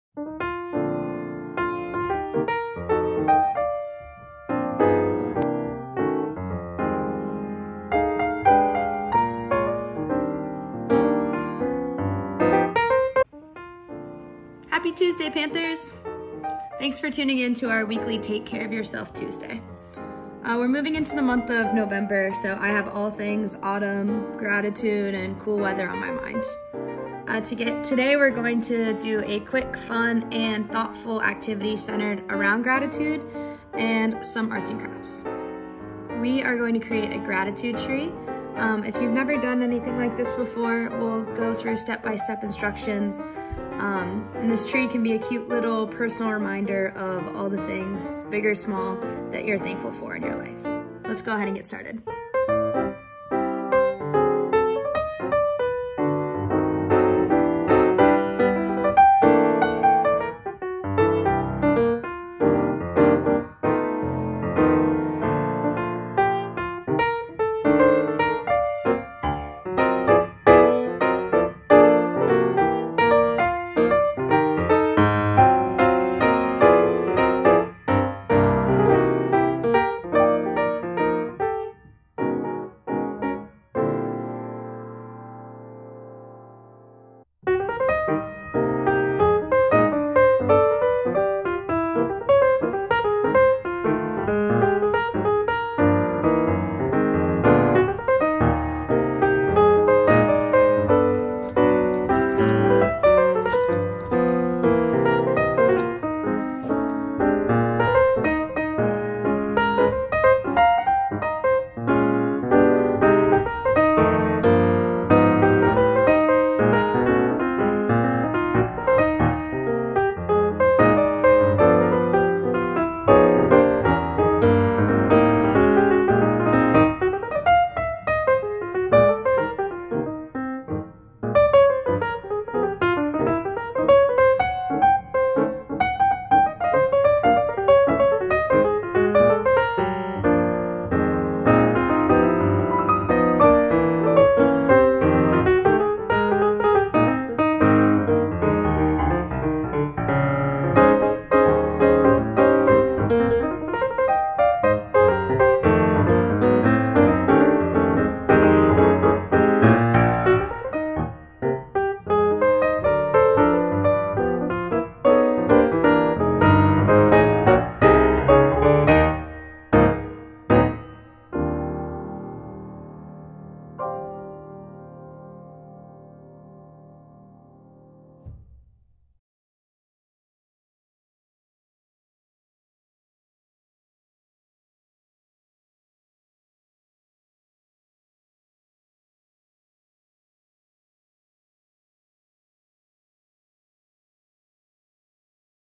TakeCareOfYouGSU: Let's make a Gratitude Tree!
This video is part of the Take Care of Yourself Tuesday video series where students learn how to use art and creativity to help them reduce stress. In this video, students will learn how to make a gratitude tree.

This video is intended for registered Georgia State University Students for educational and stress management purposes. If you are a Georgia State University student and need to speak with a Georgia State University counselor, please call the Counseling Center at .

To earn pounce rewards go to the PIN event